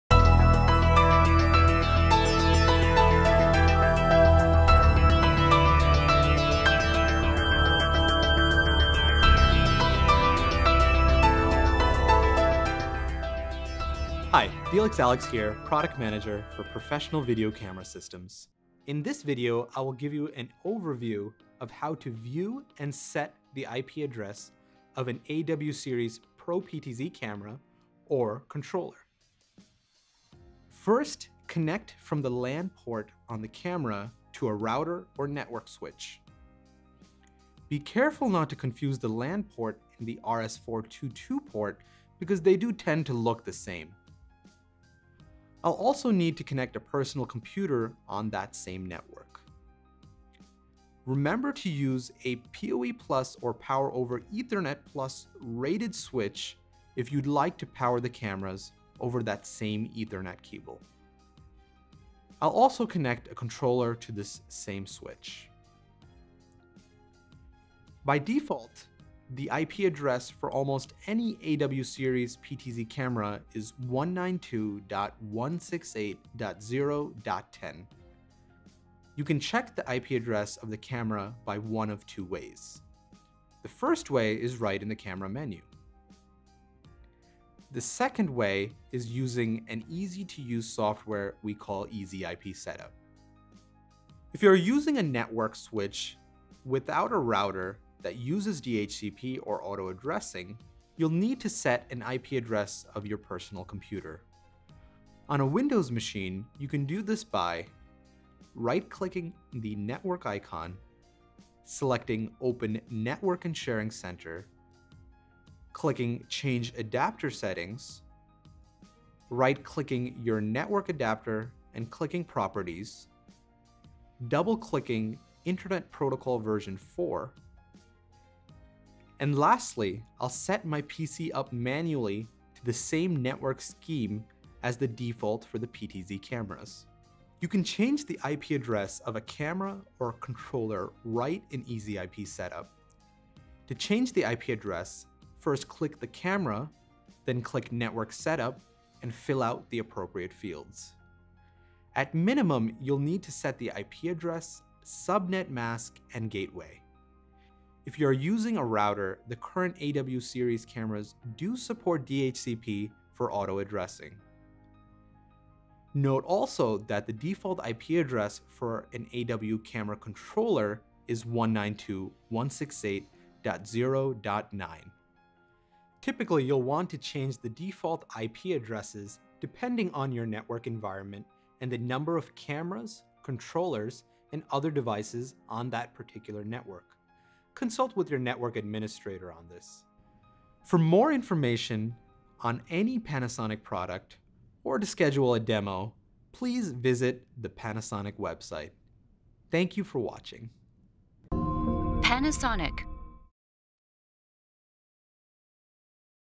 Configuring Panasonic PRO PTZ Robotic Camera IP Network Settings with EZ IP Software
Overview of how to view and set the IP address of a Panasonic AW series PTZ camera or controller.